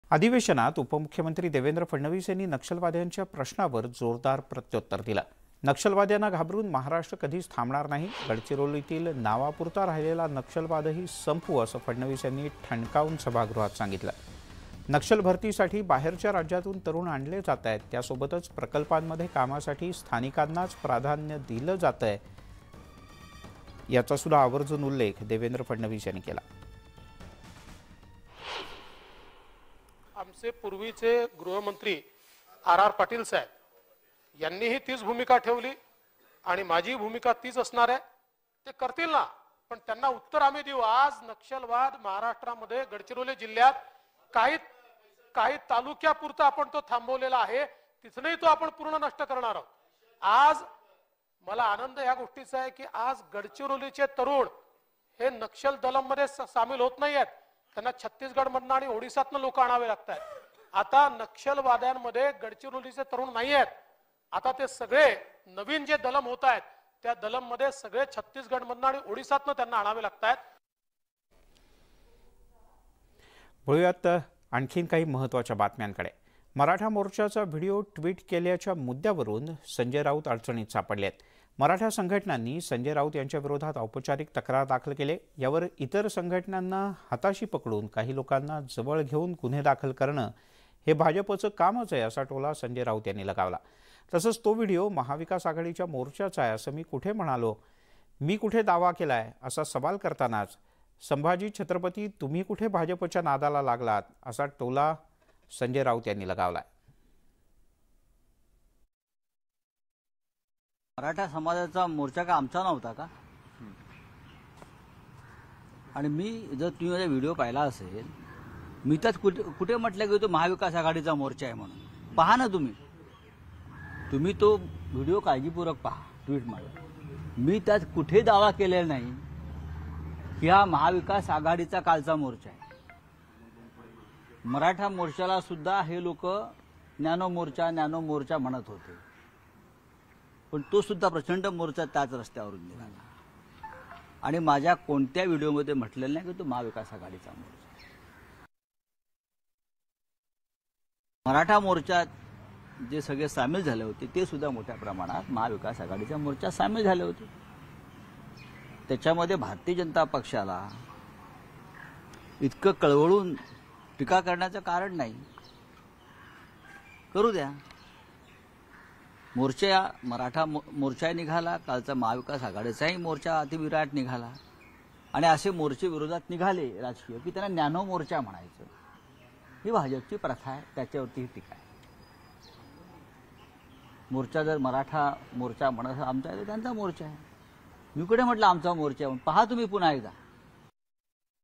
Maharashtra Winter Session 2022 : देवेंद्र फडणवीसांचं नक्षलवाद्यांना चोख प्रत्युत्तर | Marathi News
Maharashtra State Deputy Chief Minister Devendra Fadnavis has given a befitting reply to the Naxalites in the Maharashtra State Winter Session.

महाराष्ट्र राज्याचे उपमुख्यमंत्री देवेंद्र फडणवीस यांनी महाराष्ट्र राज्याच्या हिवाळी अधिवेशनामध्ये नक्षलवाद्यांना चोख प्रत्युत्तर दिले आहे.

n18oc_news